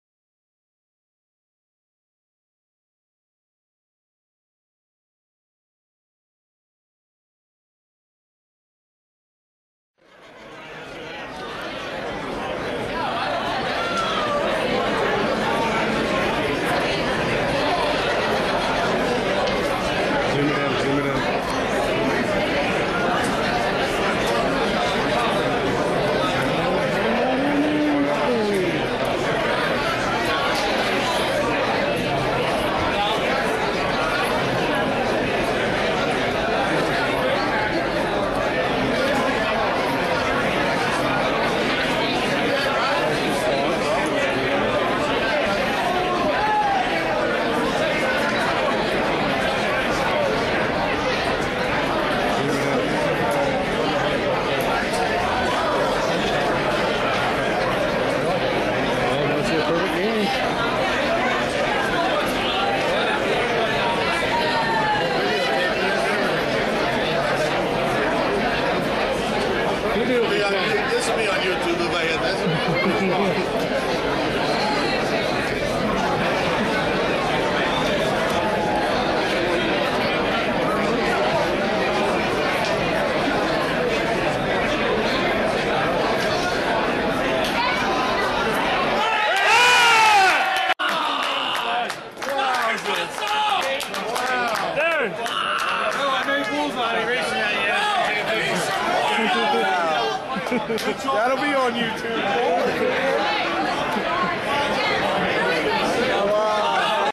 Darin Young 9 dart 501 at Rae Chesney Dart Tournament
Darin Young hit a 9 dart 501 at Rae Chesney Dart Tournament Jan-8-2010.
(recorded  by Jeff Croft)